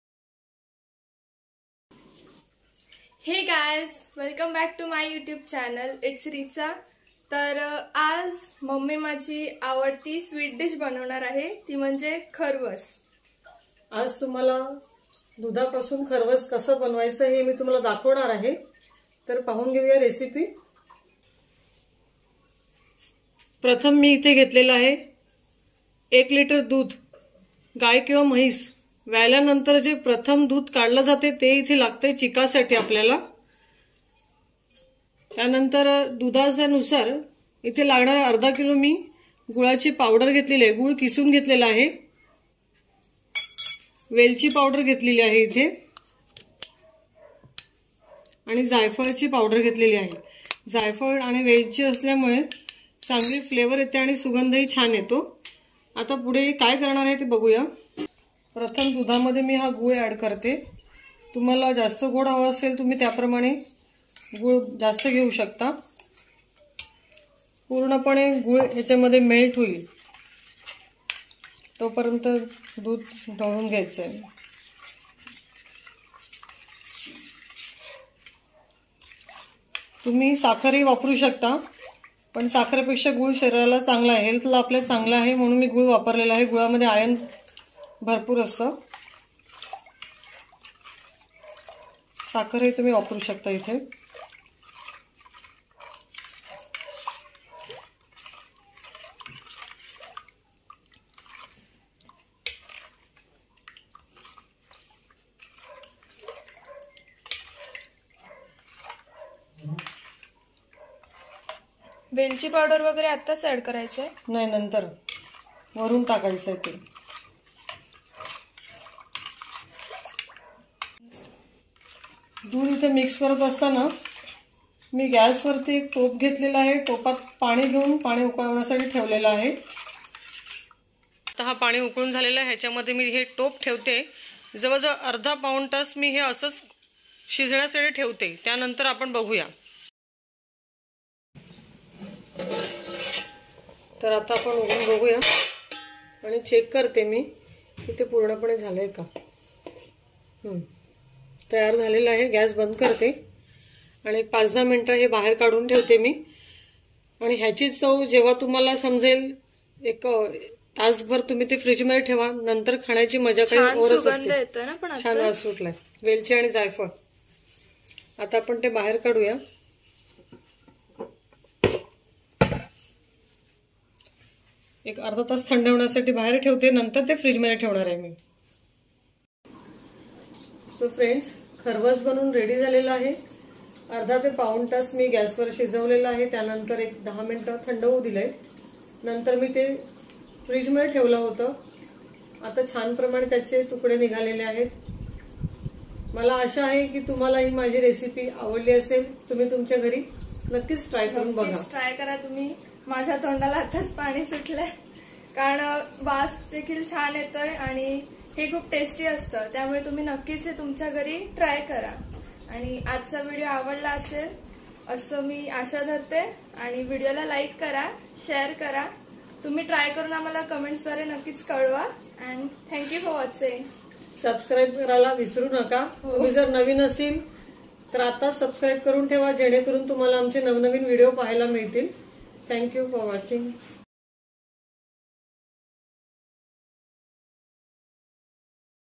How to make Kharvas | cheek | milk pudding
Kharvas is an Indian milk pudding made from cow or buffalo colostrum, the first form of milk produced within one or two days of giving birth.